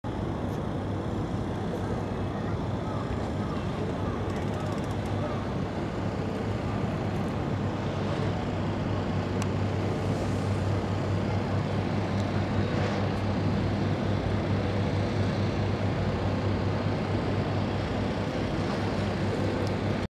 ロバート秋山のおやじの店The store of the father of the Robert Akiyama.
お笑いトリオ、ロバートの馬場と秋山は、福岡県北九州市門司区出身である。その秋山の父親が、門司レトロ地区で船上レストランを経営している。地元ではマスコミにも登場して有名である。それが、このレストラン。Comedy trio, Robert Baba and Akiyama is a born Kitakyushu, Fukuoka Prefecture Moji-ku. Father of the Akiyama, owns a shipboard restaurants in Moji retro district. Is famous also appeared in the media in the local. It is this restaurant.